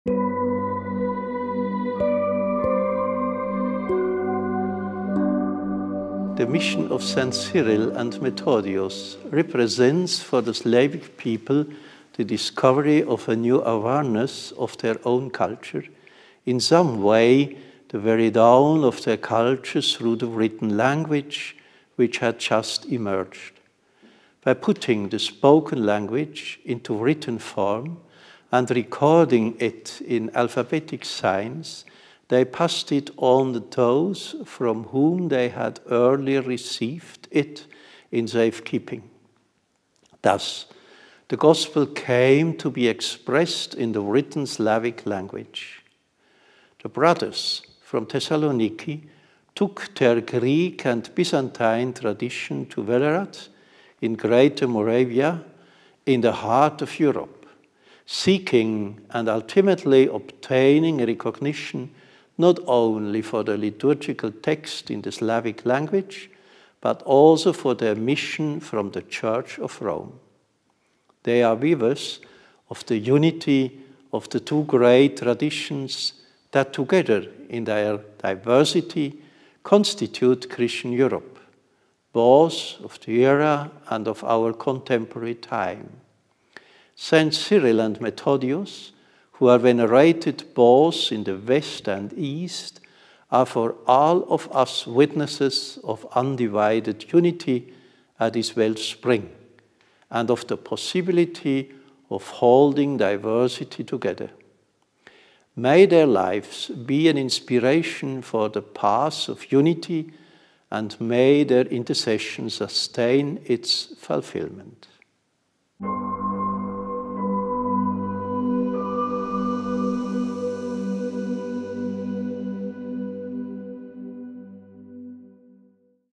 Cardinal Kurt Koch - President of the Pontifical Council for Promoting Christian Unity
Europe celebrates the proclamation of Saints Cyril and Methodius Co-Patrons of Europe by St. John Paul II on 31st December 1980. Ambassadors of several European Countries accredited to the Holy See together with some Vatican authorities share comments on our YouTube Channel on the 40th anniversary via a short video-message.

As a backdrop to the event we have kept the location where we would have originally held the talks: the Basilica of Santa Prassede in Rome. This is the site of a Greek-Byzantine monastery in the 9th Century where the two Saints stayed during their time in the Urbs.

A mission of discovery. A new alphabet as a vehicle to consolidate and circulate the most crucial message: the Holy Scriptures and the mission of the Church. Inspiring thoughts by H. Em. Cardinal Kurt Koch - President of the Pontifical Council for Promoting Christian Unity.